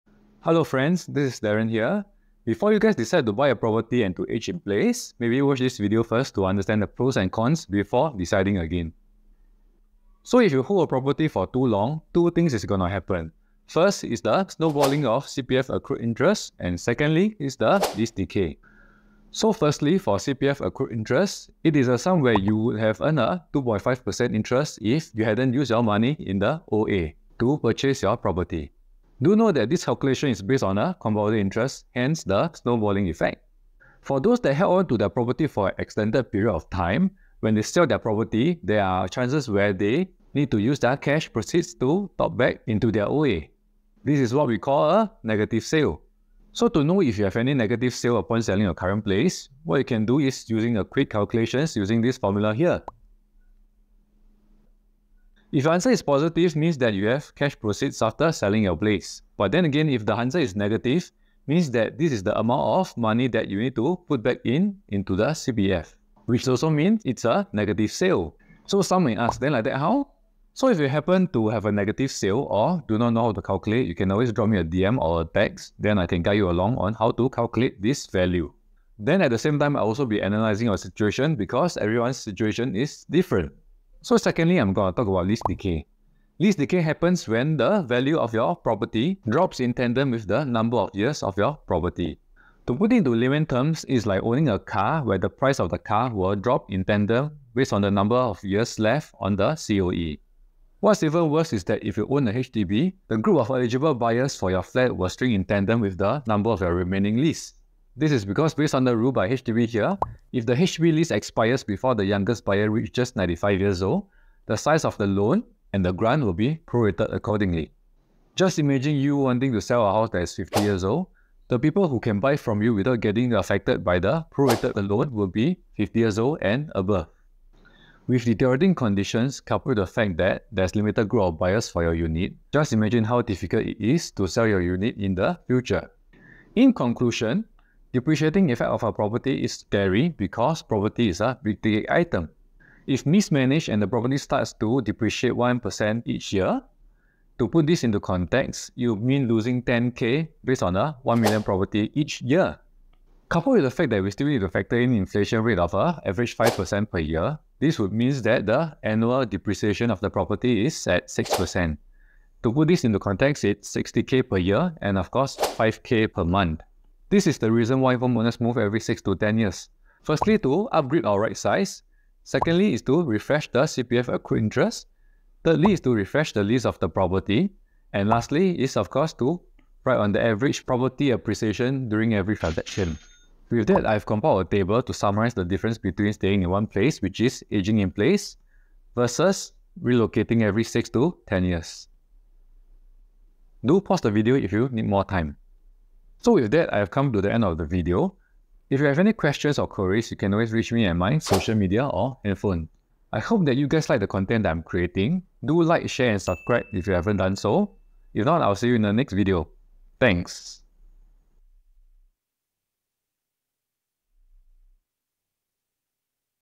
The Surprising Truth About Aging in Place in Singapore Property: What You Need to Know
Are you curious about the risk of aging in place? In this video, we'll explore the risks and benefits of aging in place, and help you decide whether or not it's the right decision for you.

Aging in place can be a great choice for seniors who are able to remain independent and live in their own homes. However, there are risks associated with aging in place, and it's important to know them before making a decision. In this video, we'll discuss the risks of aging in place and help you make an informed decision about whether or not aging in place is the right choice for you!

00:00 Introduction
00:10 Age-in-Place
00:19 Snowballing of Accrued Interest
00:34 Understanding Negative Sale
01:24 Lease Decay
02:16 Property Deprecation / Profit Erosion
03:16 Outpro